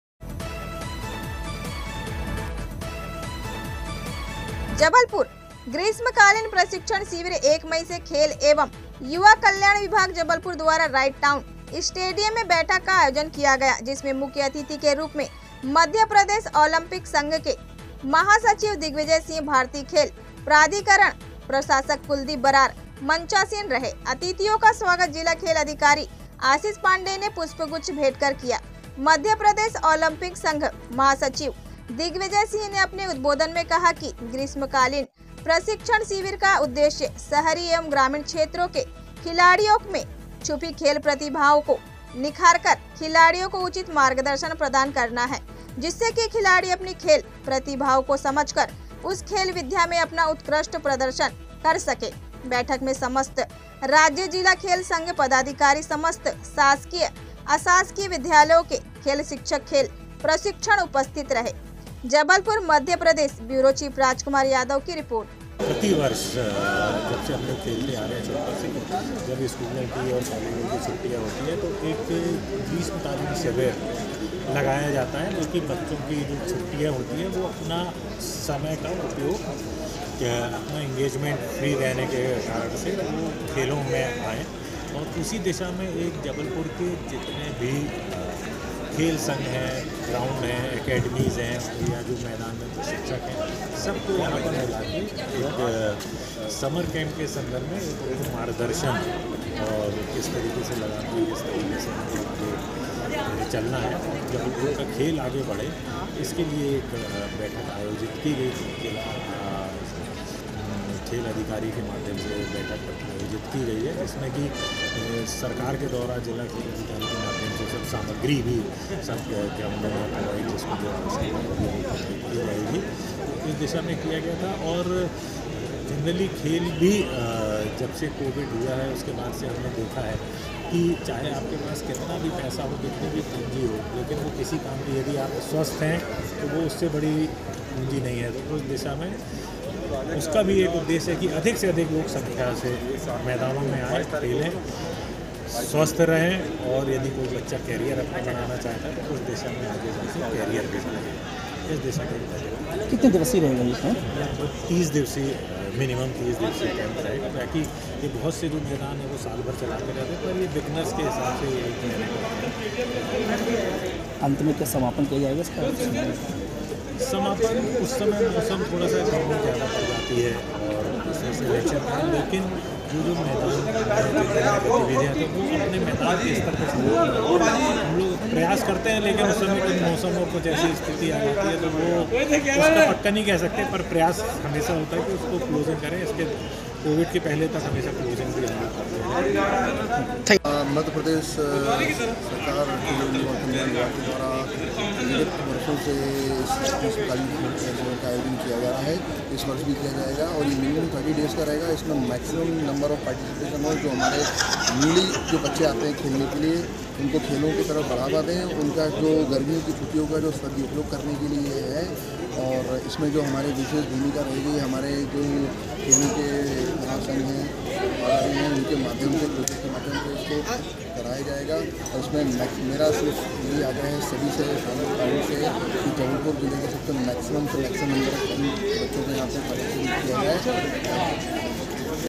ग्रीष्मकालीन प्रशिक्षण शिविर 1मई से , खेल एवं युवा कल्याण विभाग जबलपुर द्वारा राइट टाउन स्टेडियम मे
ग्रीष्मकालीन प्रशिक्षण शिविर 1मई से , खेल एवं युवा कल्याण विभाग जबलपुर द्वारा राइट टाउन स्टेडियम में बैठक का आयोजन किया गया